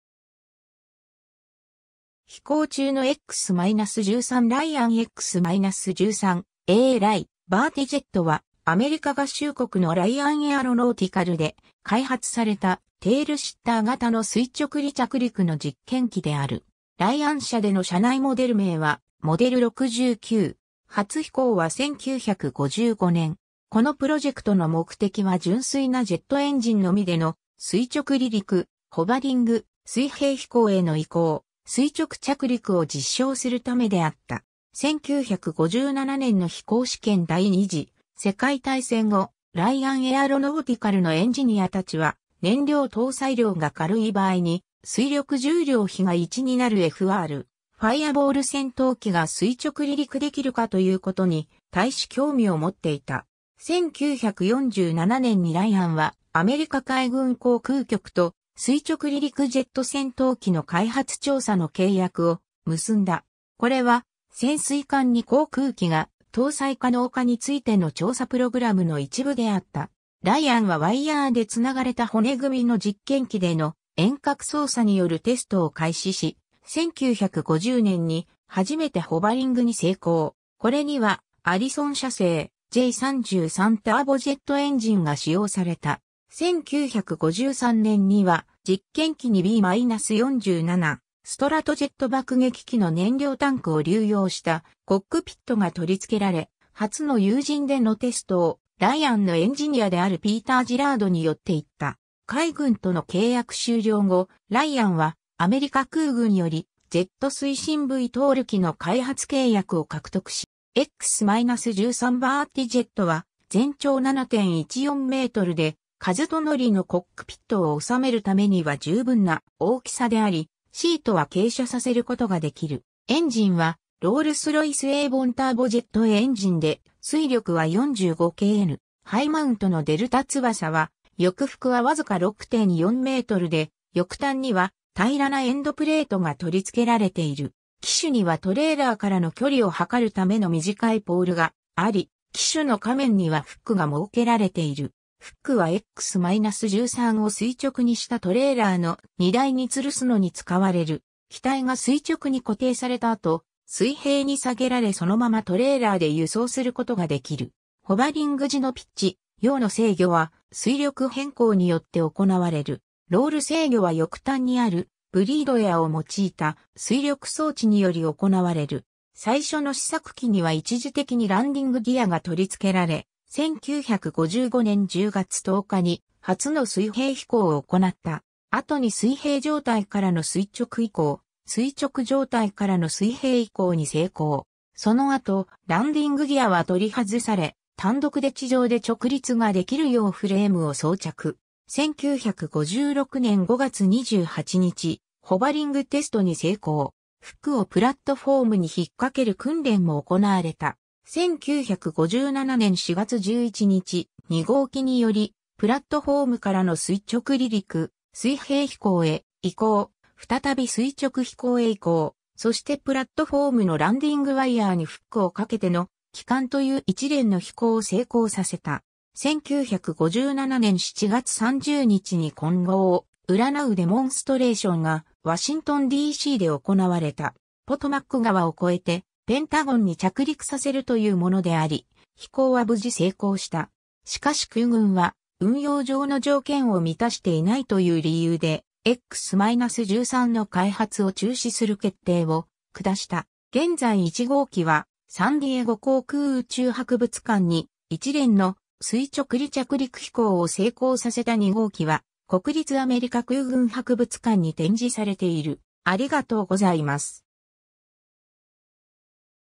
X-13 (航空機)
飛行中のX-13 ライアン X-13A-RY バーティジェット (Ryan X-13 Vertijet)は、アメリカ合衆国のライアン・エアロノーティカルで開発されたテイルシッター型の垂直離着陸（VTOL）の実験機である。
ライアン社での社内モデル名はモデル69。
初飛行は1955年。
このプロジェクトの目的は純粋なジェットエンジンのみでの垂直離陸、ホバリング、水平飛行への移行、垂直着陸を実証するためであった。
 1957年の飛行試験 第二次世界大戦後、ライアン・エアロノーティカルのエンジニアたちは、燃料搭載量が軽い場合に推力重量比が1になるFRファイアボール戦闘機が垂直離陸できるかということに対し興味を持っていた。
1947年にライアンはアメリカ海軍航空局と垂直離陸ジェット戦闘機の開発調査の契約を結んだ。
これは潜水艦に航空機が搭載可能かについての調査プログラムの一部であった。
 ライアンはワイヤーでつながれた骨組の実験機での遠隔操作によるテストを開始し、1950年に初めてホバリングに成功。
これにはアリソン社製J33ターボジェットエンジンが使用された。
1953年には実験機にB-47ストラトジェット爆撃機の燃料タンクを流用したコックピットが取り付けられ、初の有人でのテストをライアンのエンジニアであるピーター・ジラードによって行った。
海軍との契約終了後、ライアンはアメリカ空軍よりジェット推進VTOL機の開発契約を獲得し、実際に2機のプロトタイプが製作された。
 X-13ヴァーティジェットは全長7.14mで、一人乗りのコックピットを収めるためには十分な大きさであり、シートは傾斜させることができる。
エンジンはロールス・ロイス エイヴォン ターボジェットエンジンで推力は45kN。
ハイマウントのデルタ翼は翼幅はわずか6.4mで、翼端には平らなエンドプレートが取り付けられている。
機首にはトレーラーからの距離を測るための短いポールがあり、機首の下面にはフックが設けられている。
フックはX-13を垂直にしたトレーラーの荷台（着陸プラットフォーム）に吊るすのに使われる。
機体が垂直に固定された後、水平に下げられそのままトレーラーで輸送することができる。
ホバリング時のピッチ、ヨーの制御は推力偏向によって行われる。
ロール制御は翼端にあるブリードエアを用いた推力装置により行われる。
 最初の試作機（USAF 54-1619）には一時的にランディングギアが取り付けられ、1955年10月10日に初の水平飛行を行った。
後に水平状態からの垂直移行、垂直状態からの水平移行に成功。
その後ランディングギアは取り外され、単独で地上で直立ができるようフレームを装着。
1956年5月28日ホバリングテストに成功。
フックをプラットフォームに引っ掛ける訓練も行われた。
 1957年4月11日、2号機（USAF 54-1620）によりプラットフォームからの垂直離陸、水平飛行へ移行、再び垂直飛行へ移行、そしてプラットフォームのランディングワイヤーにフックを掛けての帰還という一連の飛行を成功させた。
 1957年7月30日に今後を占うデモンストレーションがワシントンD.C.で行われた。
ポトマック川を越えてペンタゴンに着陸させるというものであり、飛行は無事成功した。
しかし空軍は、運用上の条件を満たしていないという理由でX-13の開発を中止する決定を下した。
 現在1号機はサンディエゴ航空宇宙博物館に、一連の垂直離着陸飛行を成功させた2号機は国立アメリカ空軍博物館に展示されている。